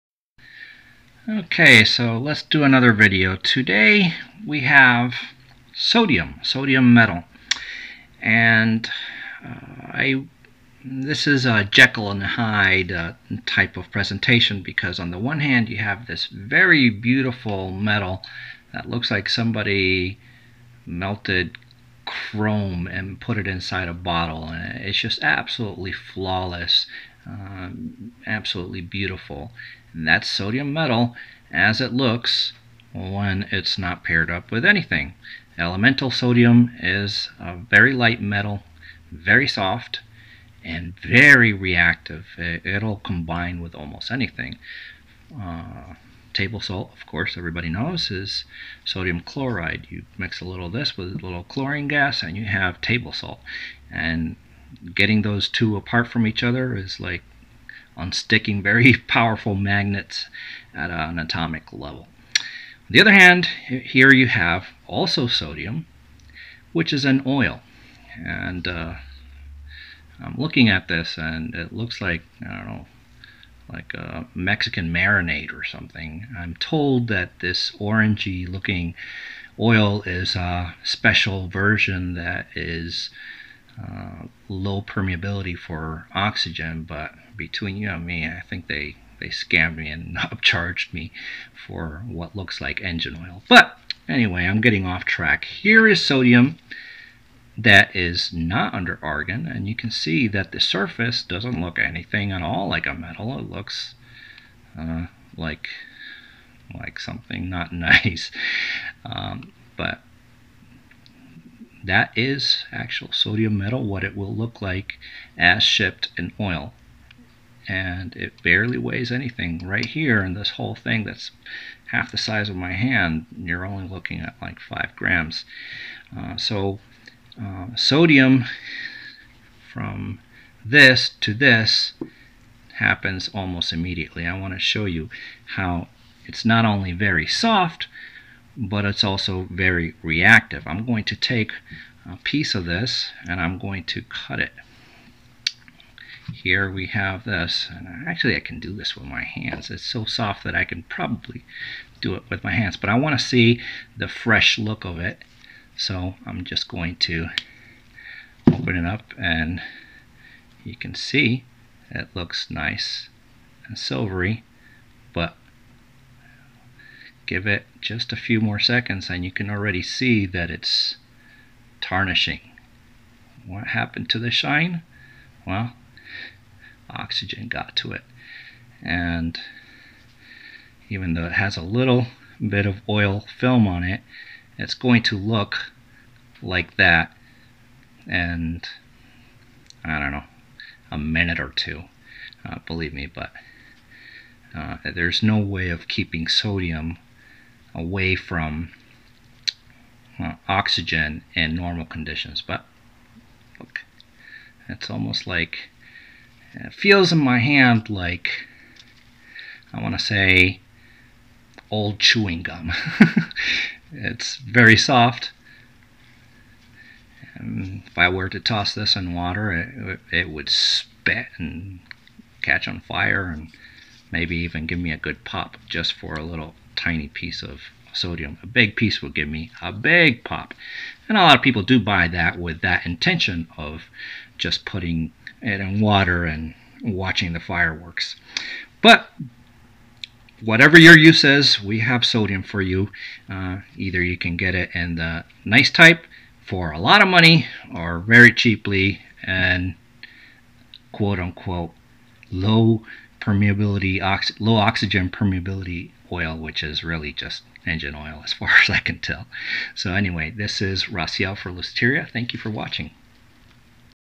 What does sodium metal look like?
99.95% pure sodium metal in oil and 99.99% pure sodium metal in glass ampules under argon gas from Luciteria

The element we commonly associate with salt - and keeping an eye on our diet so as to not get too much to save us from high blood pressure - is to most just a chemical with a name with relatively few being aware that it is a shiny metal that looks just like silver. Except it's so soft that it can be parted with a fork. And on exposure to air that silver sheen turns to muck in a matter of seconds. In water, as you can see thanks to a million YouTube videos, it quickly goes from hissing to a fiery *KABOOM*. In fact, it is so reactive that even in mineral oil, the chemical industry standard for inert storage, still has enough stray atoms of something-other-than-oil that they're enough to spoil that beautiful finish. Our high-grade mineral oil coats this small sample to stop the corrosion to the thinnest possible depth preserving the bulk of the specimen.